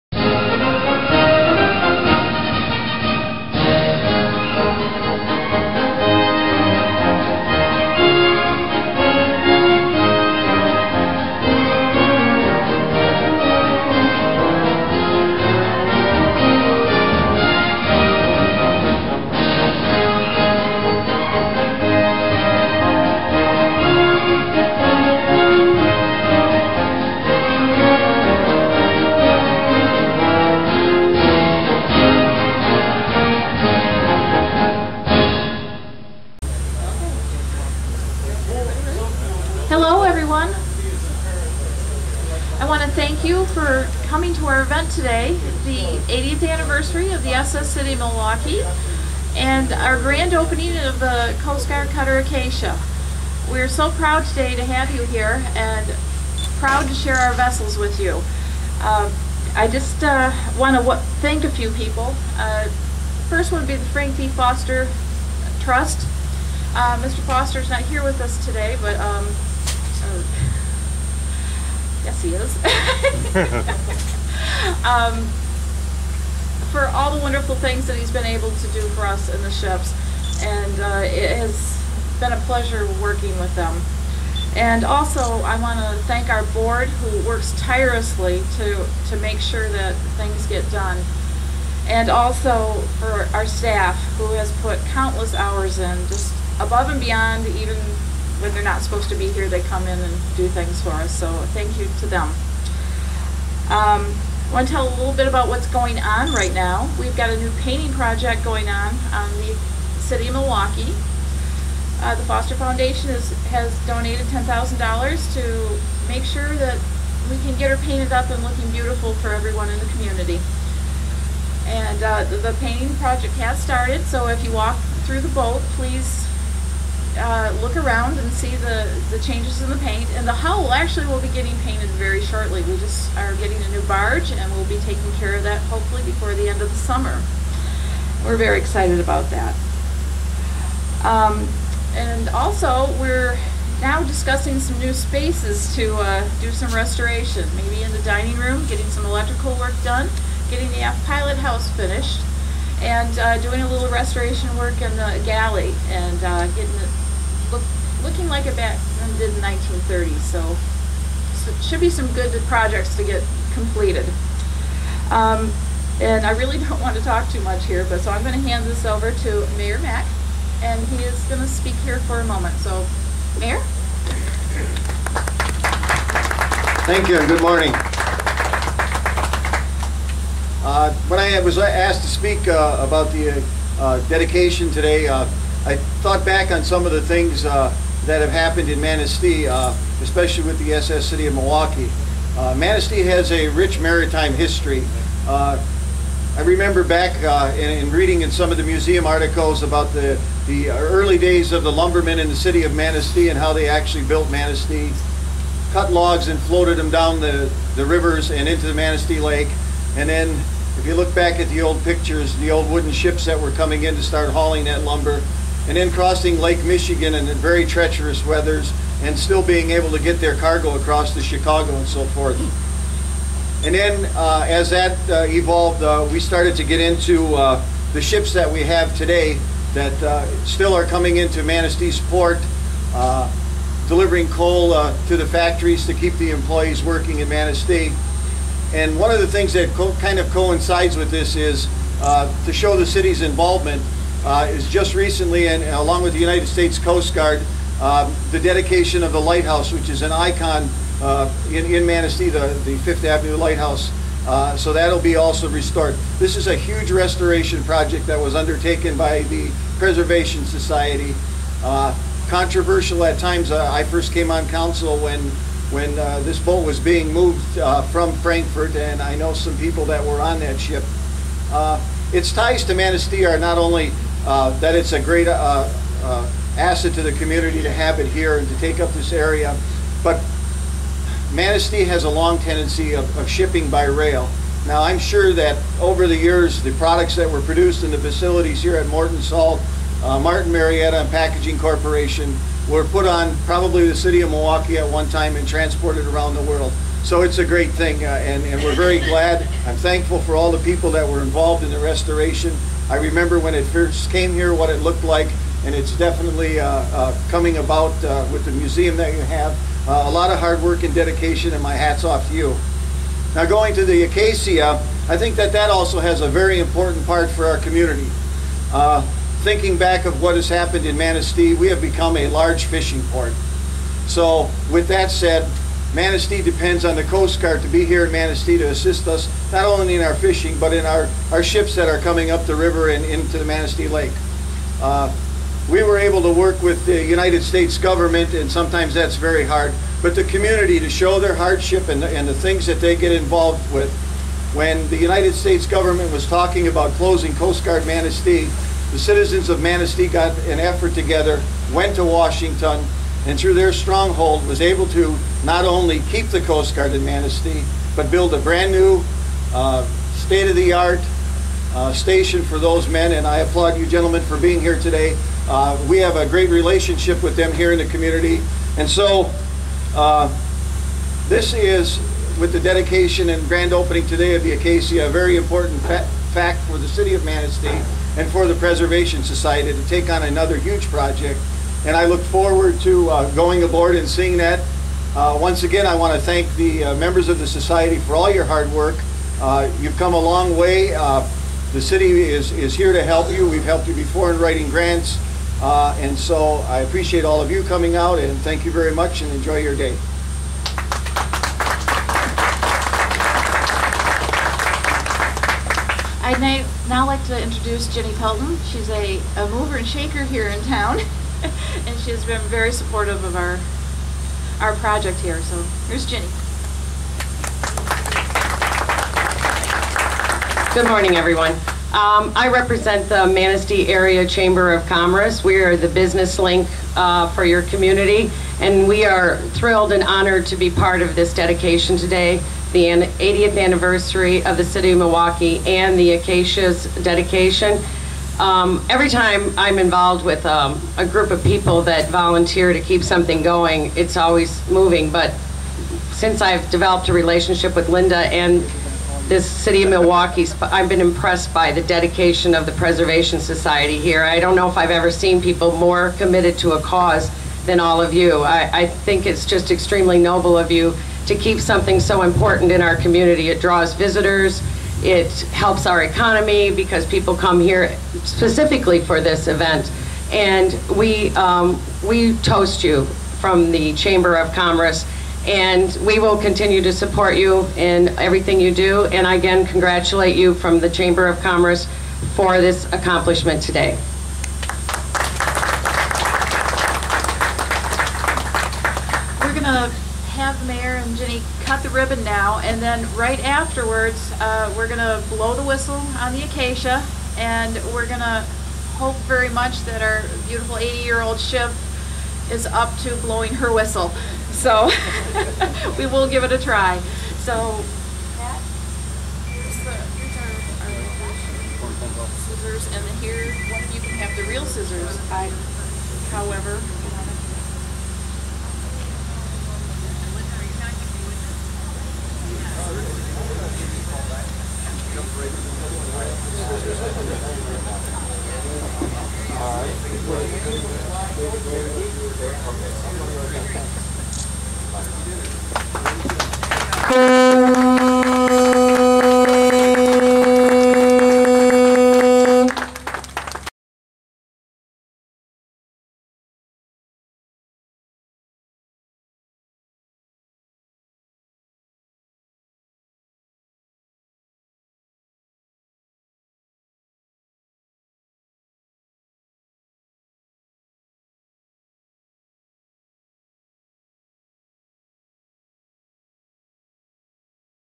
USCGC Acacia Dedication Video.wmv
The dedication ceremony of the USCGC Acacia as a museum ship in Manistee Michigan - August 13th 2011.